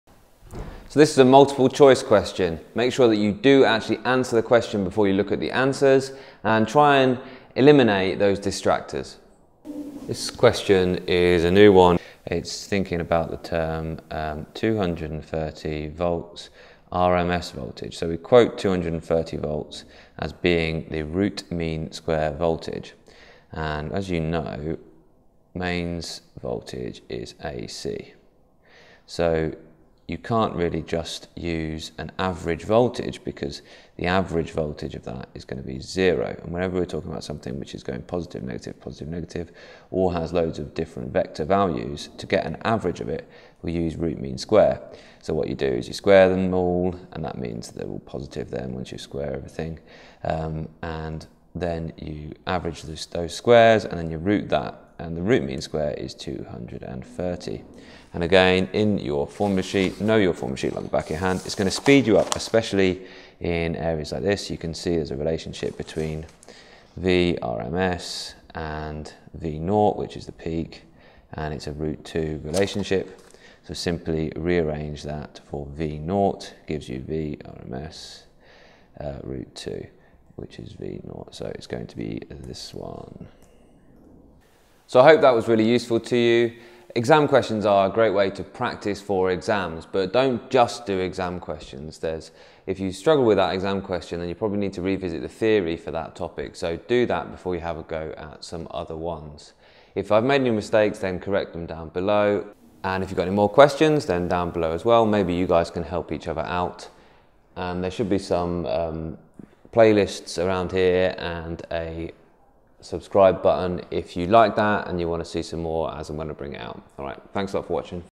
RMS Voltage - A Level Physics Exam Practice Question - Multiple Choice
Root Mean Square Volatage (RMS Voltage) is how we discuss the "average" voltage for an AC supply.  This is a new part of the new A Level.  But this is a relatively straightforward question in which you have to show how to calculate the peak voltage.

Practice exam questions are a really good way to practice for exams, but don't make it all you do.  The new A Levels are really challenging and require you to have an excellent knowledge of the A Level Syllabus as a whole, and be able to draw upon multiple parts of it, even within one question!